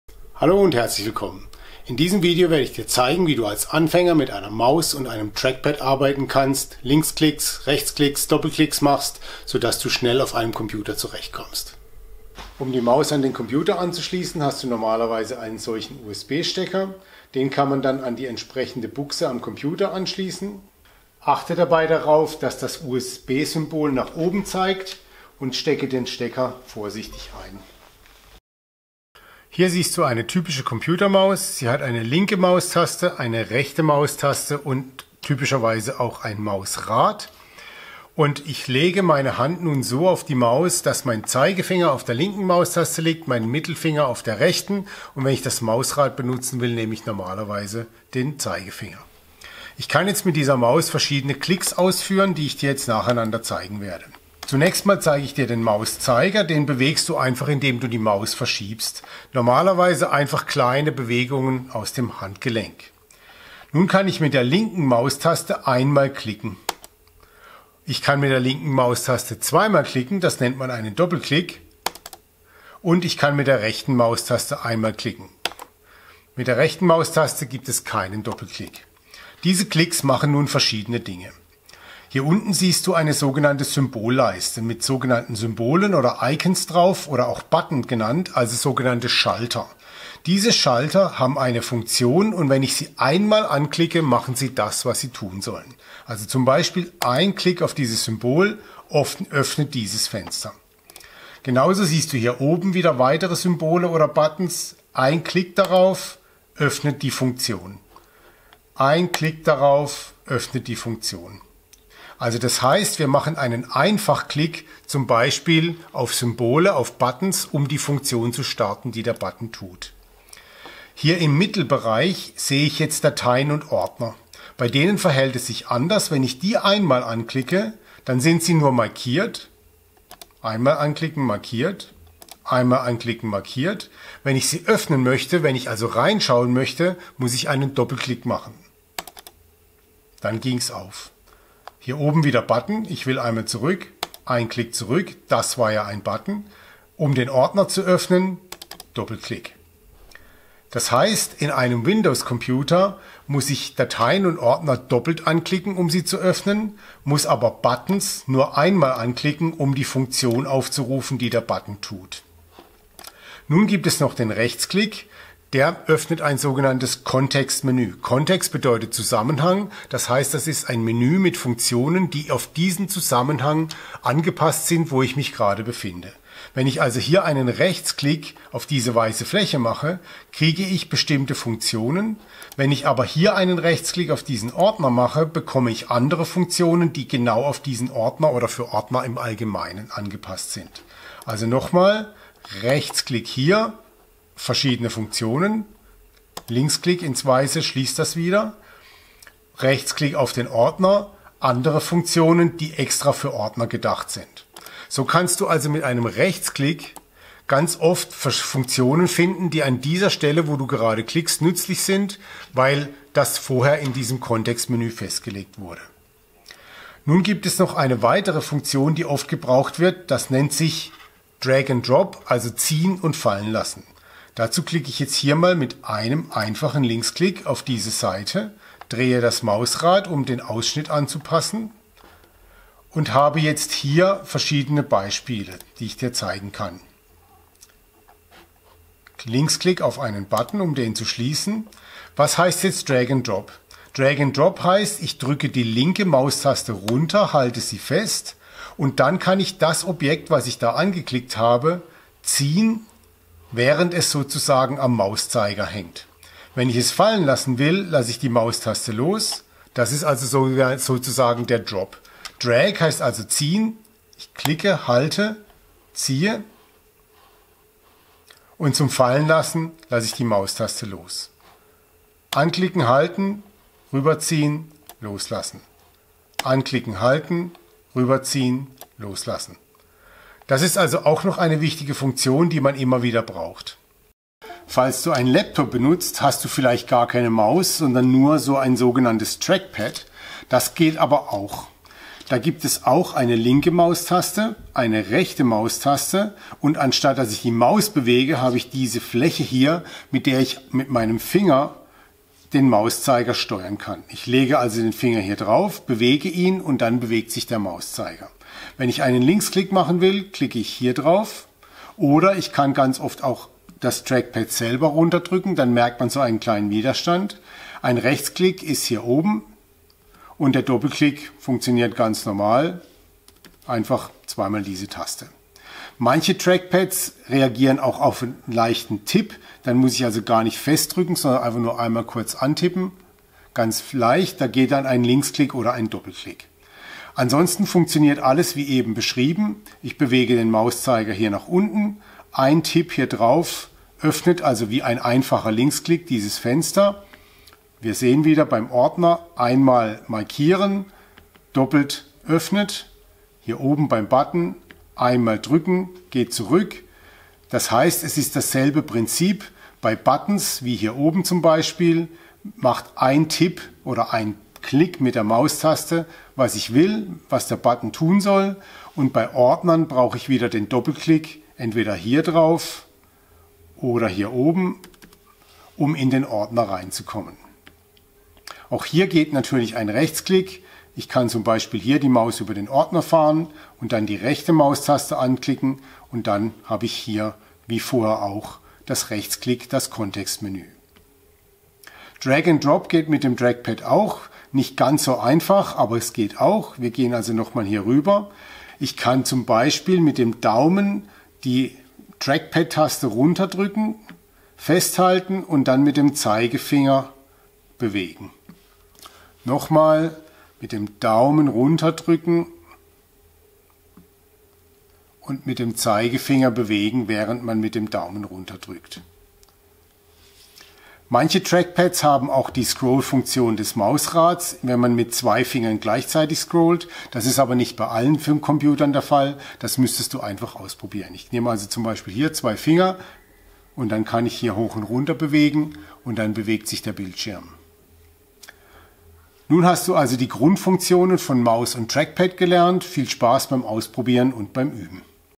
Maus und Trackpad richtig bedienen – Einfache Anleitung für Anfänger
Ich zeige dir, wie du mit der Computermaus und dem Trackpad an deinem Laptop zurecht kommst. Linksklick, Doppelklick, Rechtsklick und Drag and Drop werden erklärt und gezeigt.